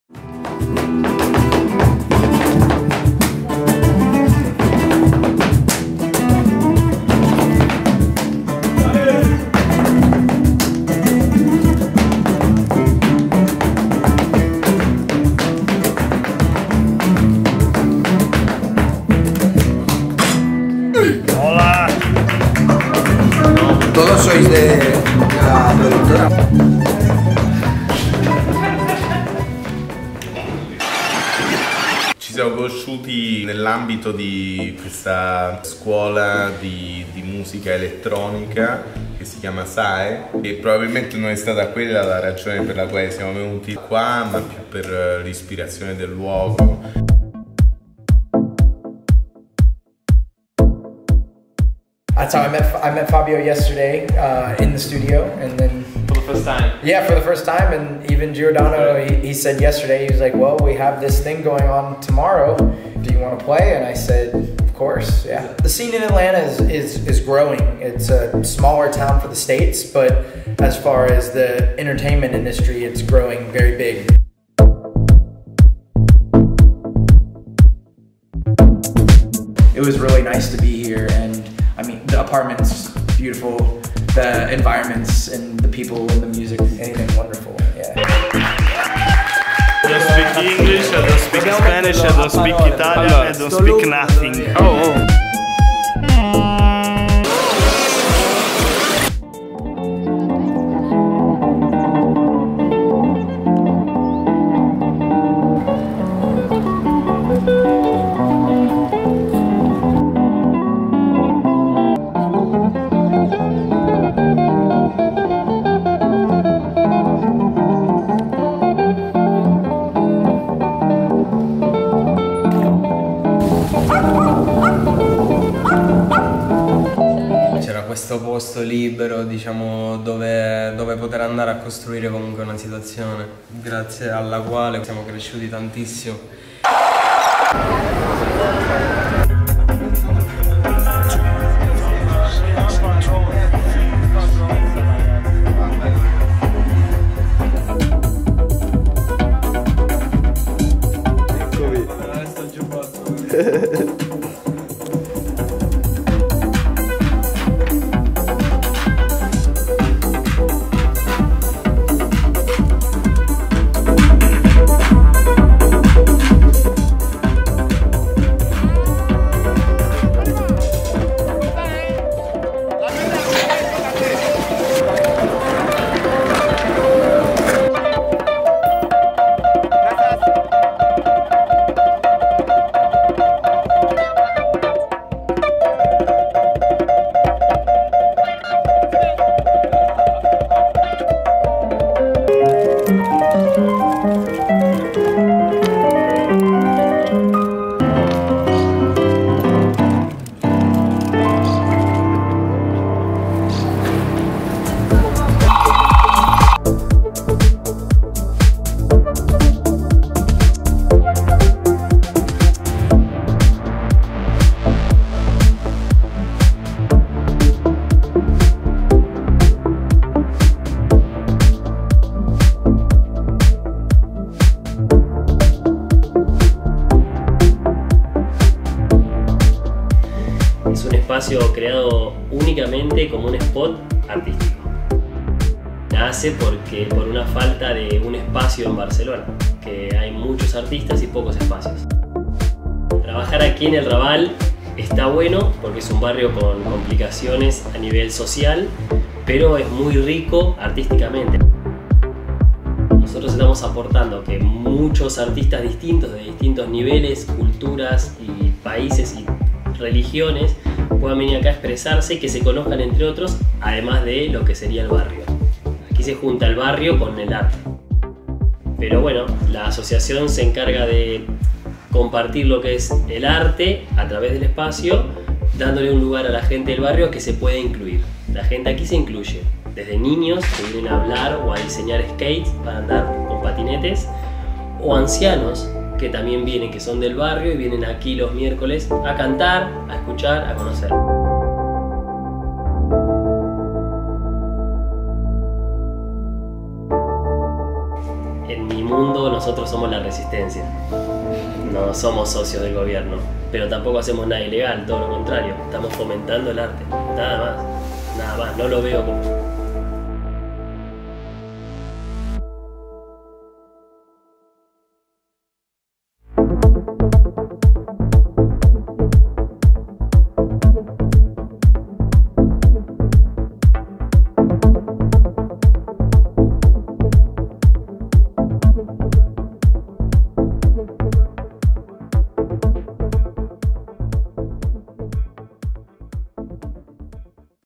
CineskaOff - MiMundo (Barcelona)
Tra le vie di Barcelona, artisti di strada ed eventi culturali, abbiamo incontrato Fabio Battaglia, Giordano Franchetti, Skybison, MOD.1 e infine Ramiro proprietario dell'associazione Old School Barcelona situata nel quartiere storico "Raval".

Durante la nostra permanenza abbiamo voluto raccontare, in maniera quanto più genuina possibile, di realtà indipendenti come la nostra.
Il resto, ve lo facciamo dire dal video.

Un ringraziamento speciale ad Agusti Zuriguel Ariaga per averci permesso di realizzare un private event a Palau Zuriaga.

Diretto da : Alessio Consoli e Simone Straq
Editing: Alessio Consoli
Subtitles: Giovanni Spadola
Event Management: Davide Anania